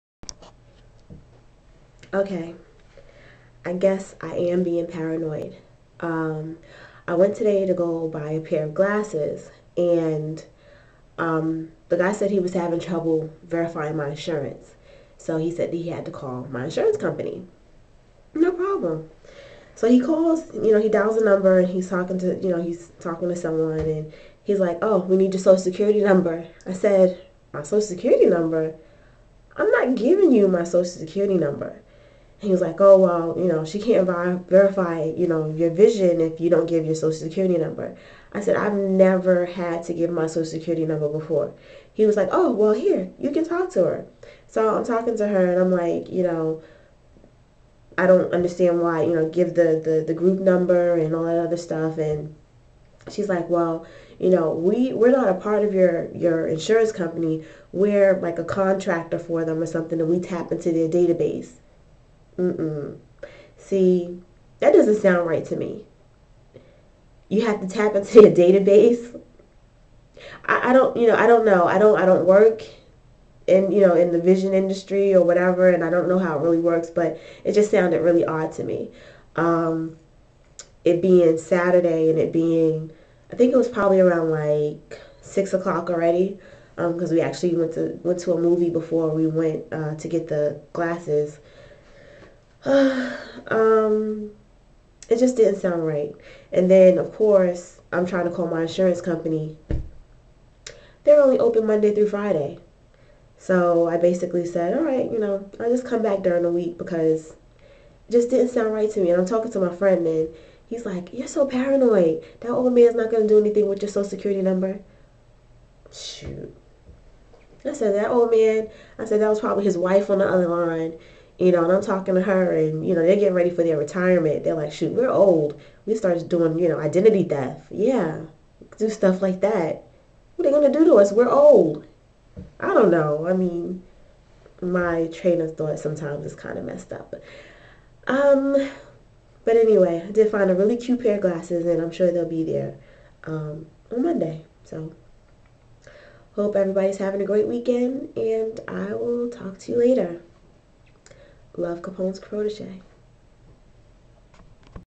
Am I Just Paranoid?!?!
Why do I need to give my social security number to buy a pair of glasses?